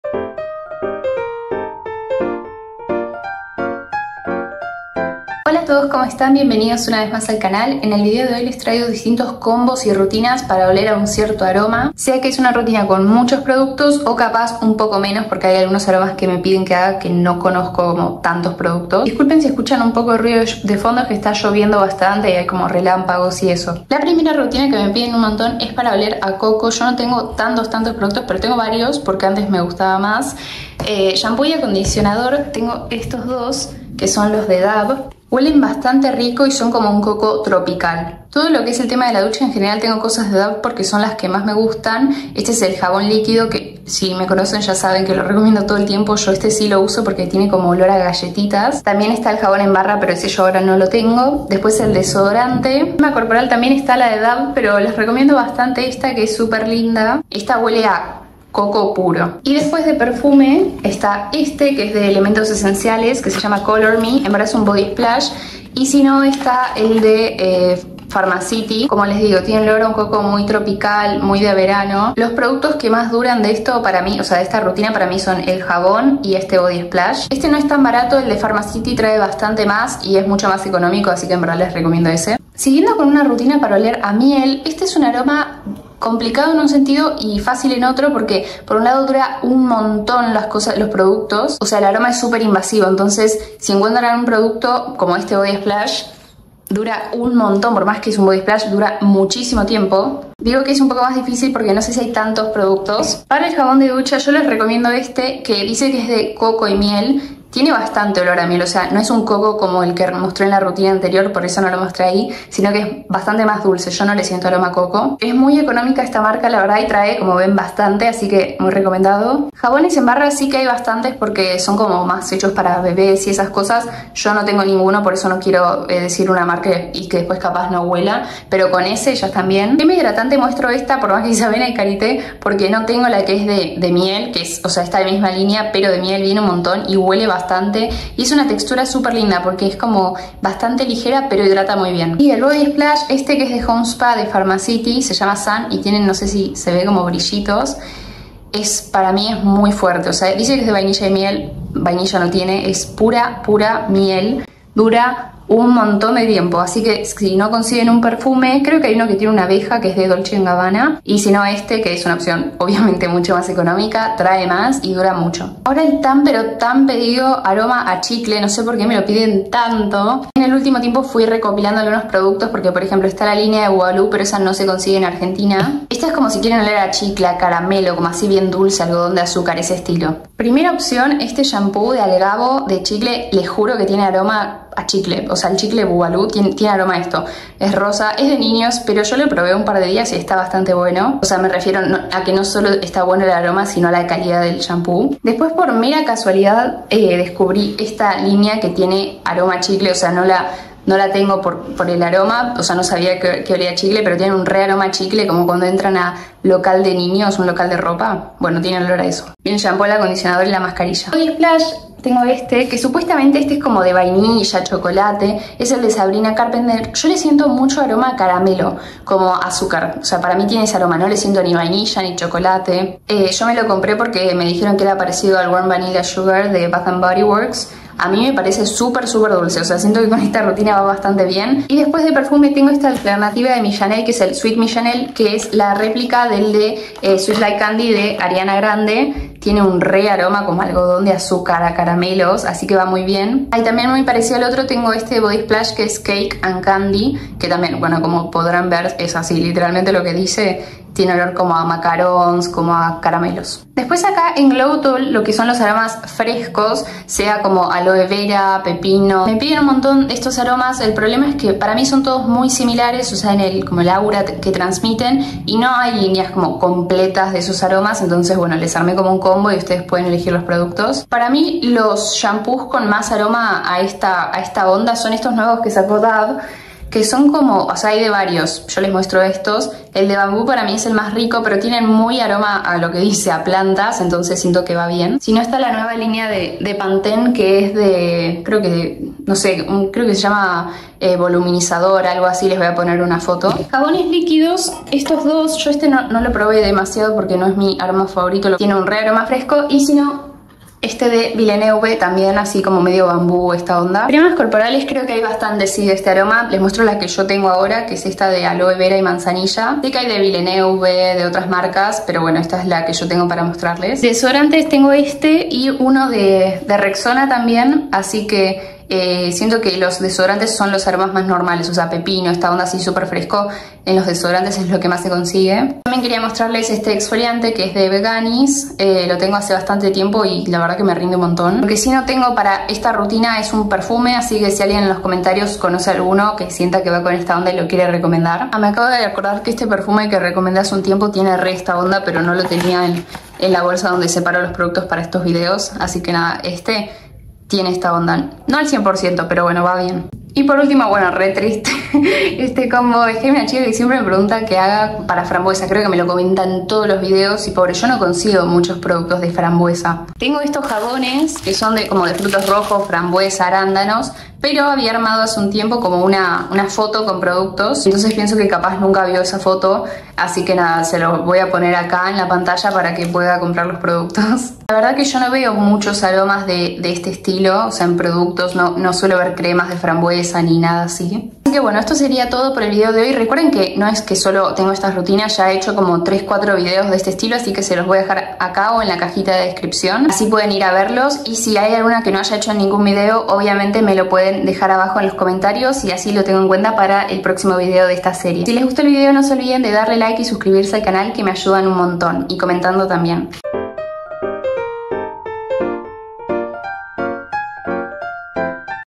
COMBOS & PRODUCTOS para OLER RICO 🥥🍓🍯🥒🥑🥝🍭🍬
Hola! Bienvenidos al canal! En el video de hoy les traigo otra parte de la serie de COMBOS Y PRODUCTOS PARA OLER RICO, algunos aromas ya los había hecho, pero ahora tengo nuevos productos para compartirles. Déjenme en los comentarios otros aromas que aún no haya hecho y que les gustaría ver!